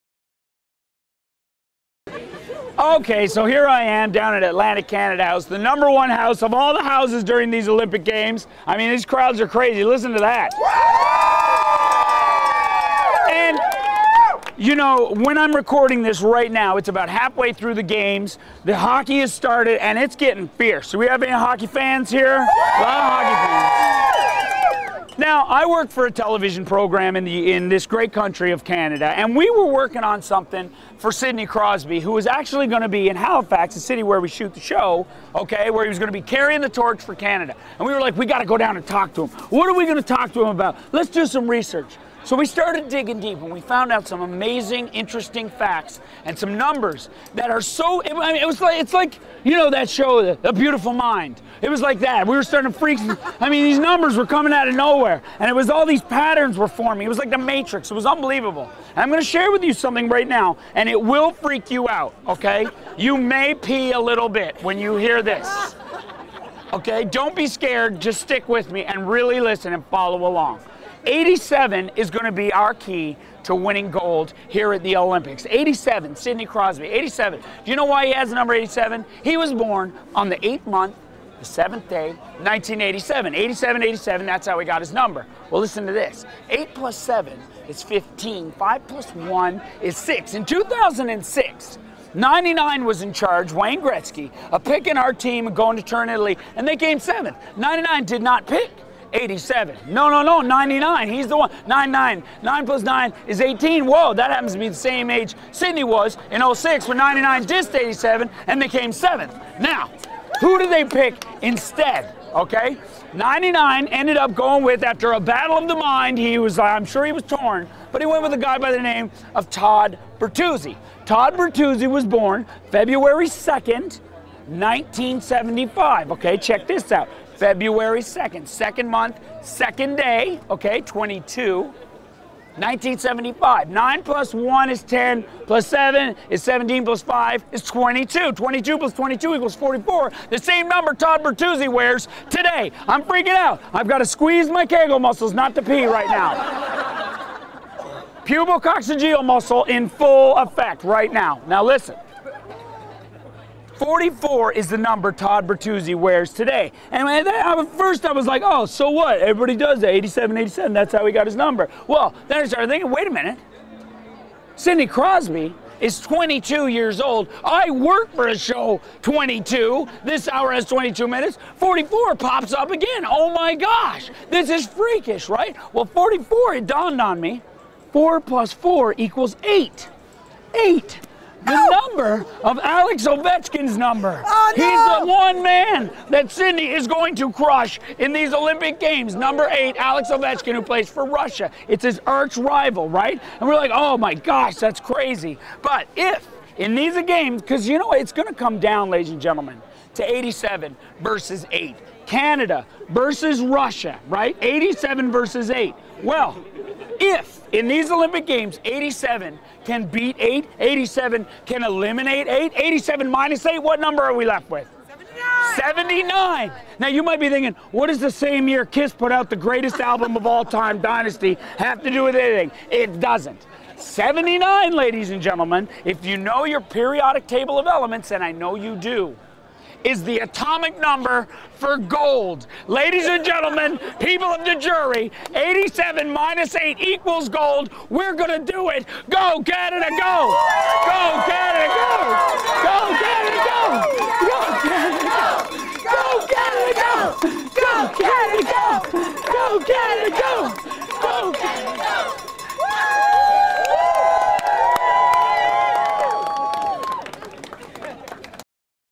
SHAUN MAJUMDER.mov
Shaun Majumder delivers his hockey numerology rant on why Canada is destined to win gold in hockey at the 2010 Winter Games.